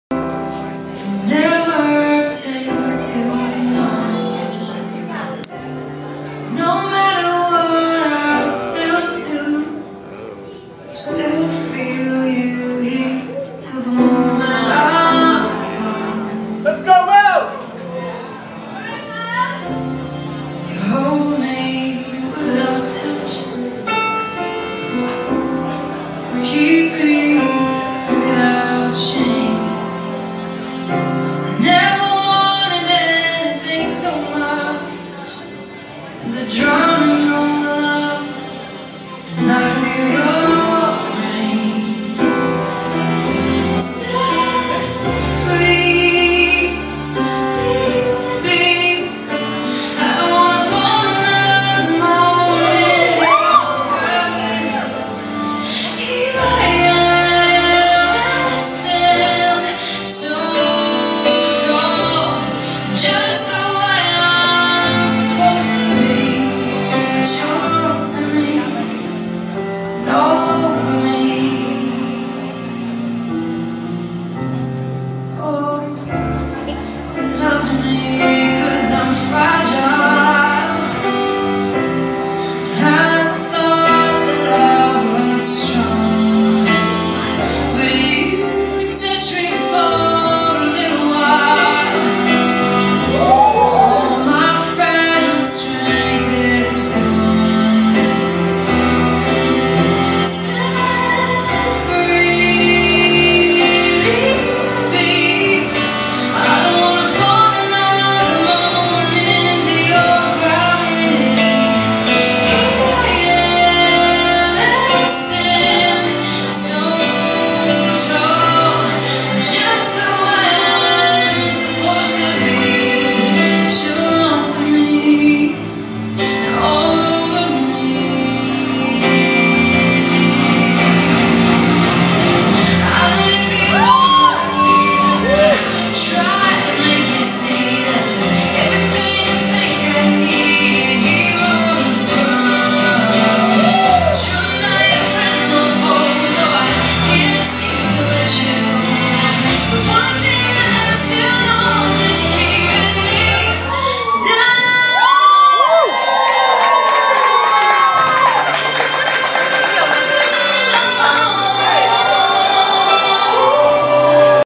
KANVAS DANCE SHOWCASE YBOR,FL
Sometimes when you want to express yourself dance can say it all. It was amazing see this right before my eyes .